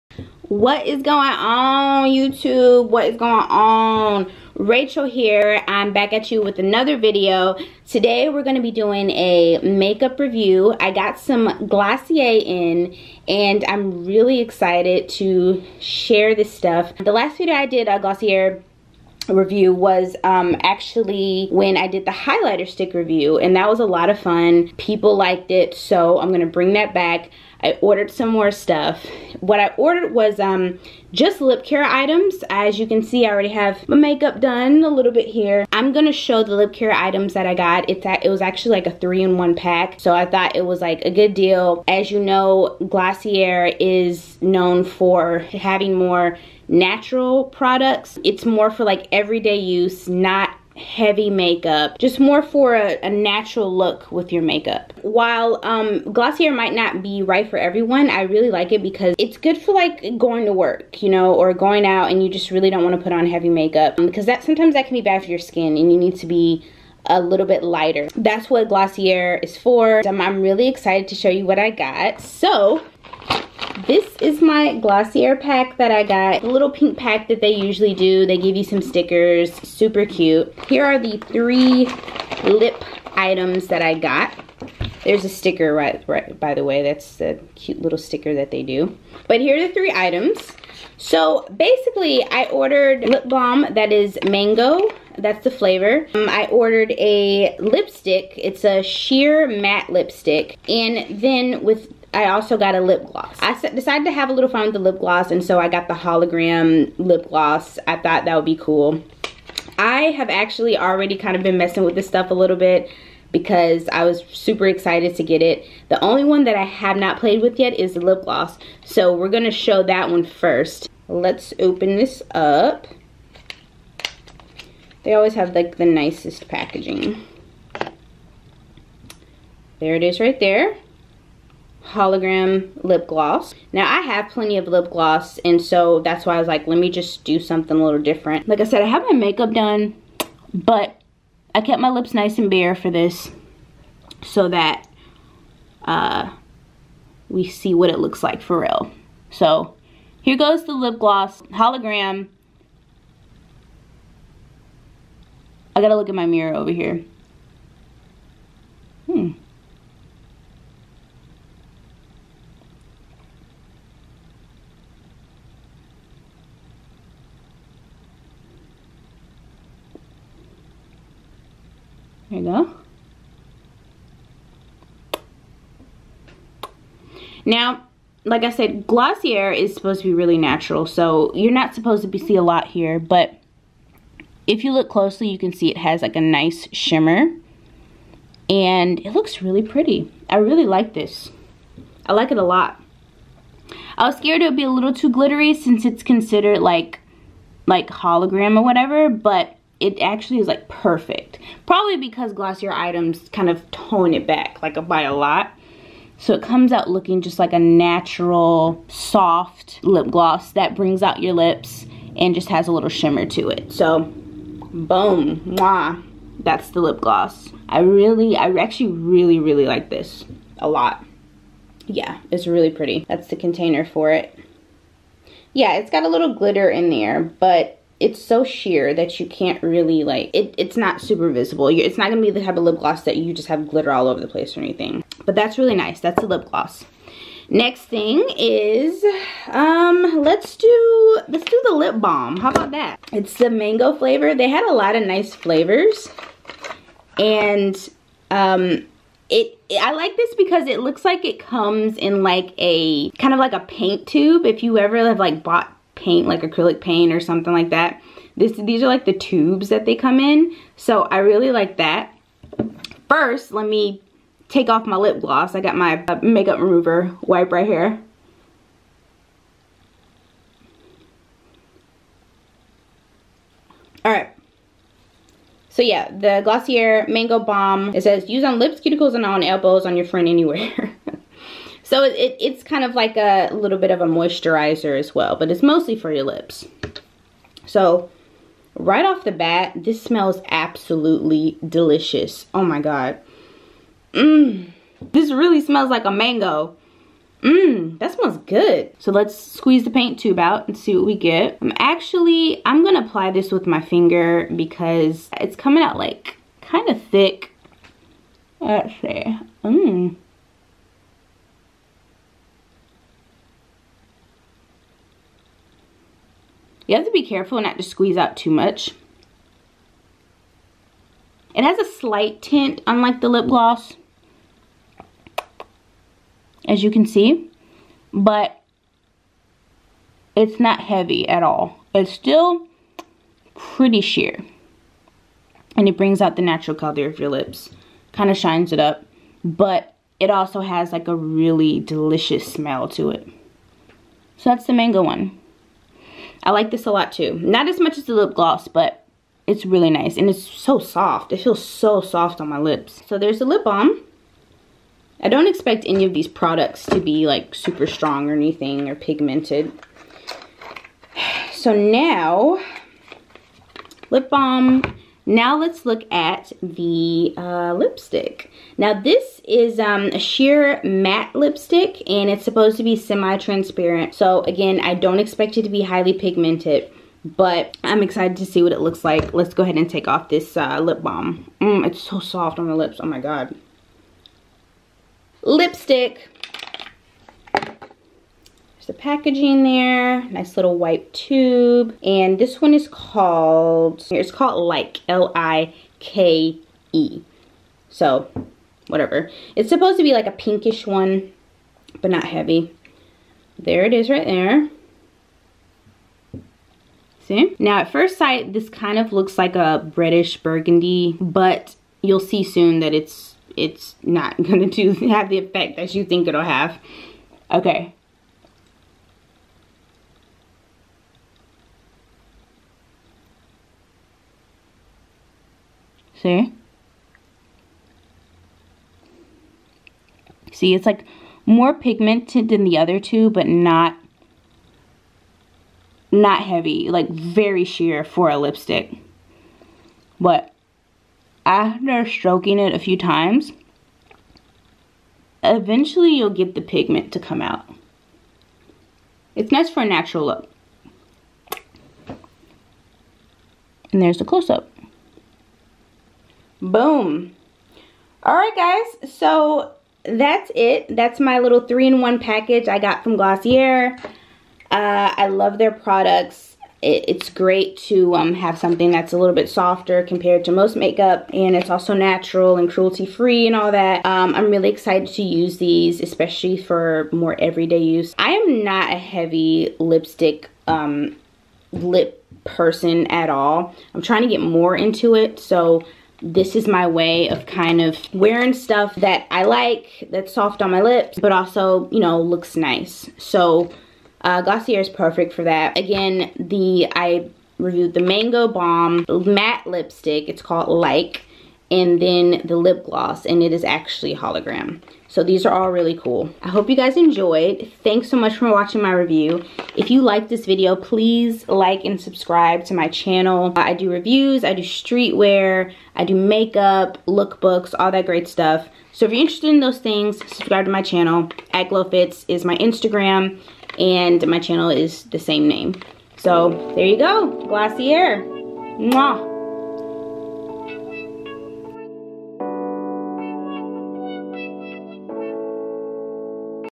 GLOSSIER LIP CARE HAUL // Subtle Lipstick // The Core Lip Collection
Just got in some lovely flavors of lipstick, lip balm, and lip gloss from Glossier! Watch as I try them on...